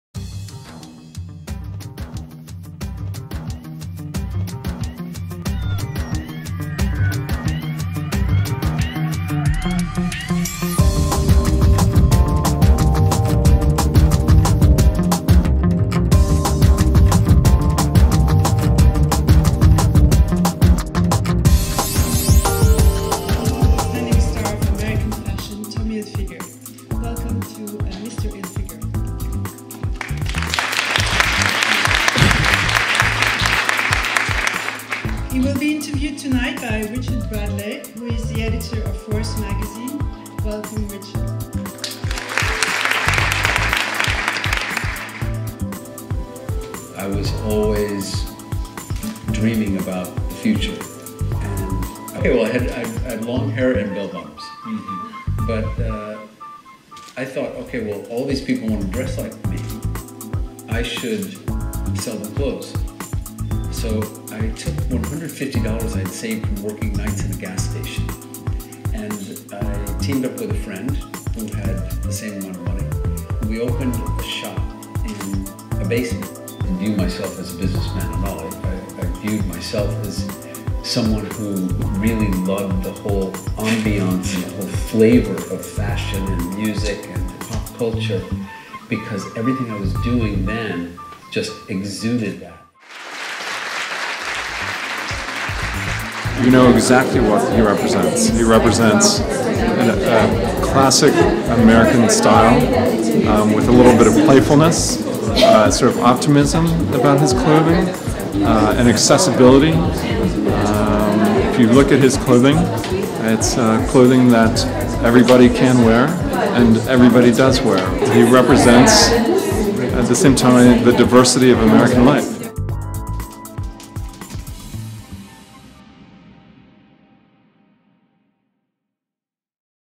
FIAF's 2010 Fashion Talks: Tommy Hilfiger
On March 3, the French Institute Alliance Francaise (FIAF) in NYC welcomed Tommy Hilfiger to Florence Gould Hall as part of its annual series of Fashion Talks